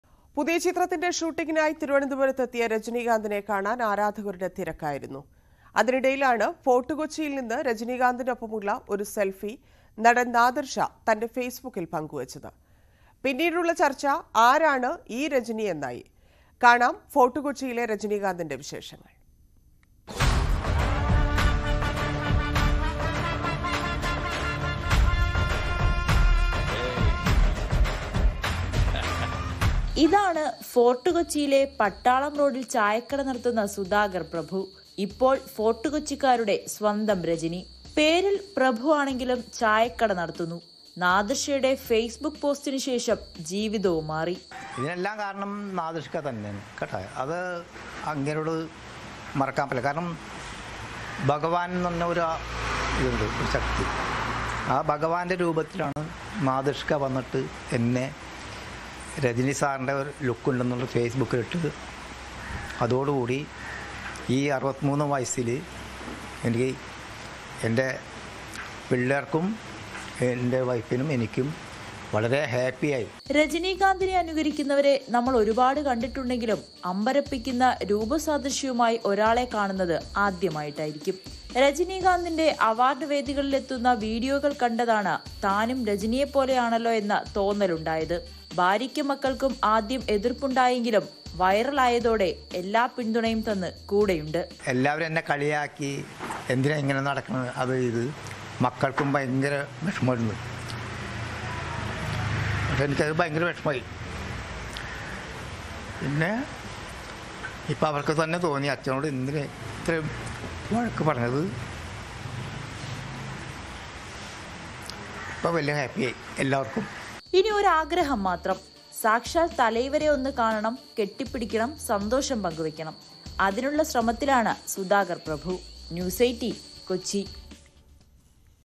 ഫോർട്ട് കൊച്ചിയിലെ രജനീകാന്ത് ! ചിത്രം പങ്കുവച്ച് നാദിർഷ | Rajani Kanth | Nadirsha
പുതിയ ചിത്രത്തിന്റെ ഷൂട്ടിങിനായി Thiruvananthapuramത്തെത്തിയ Rajani Kanthനെ കാണാൻ ആരാധകരുടെ തിരക്കായിരുന്നു. അതിനിടെയിലാണ് Fort Kochiയിൽ നിന്നും Rajani Kanthനൊപ്പമുള്ള ഒരു സെൽഫി നടൻ Nadirsha തന്റെ ഫേസ് ബുക്കിൽ പങ്കുവച്ചത്‌. പിന്നീടുള്ള ചർച്ച ആരാണ് ഈ Rajani എന്നായി. കാണാം Fort Kochiയിലെ Rajani Kanthന്റെ വിശേഷങ്ങൾ.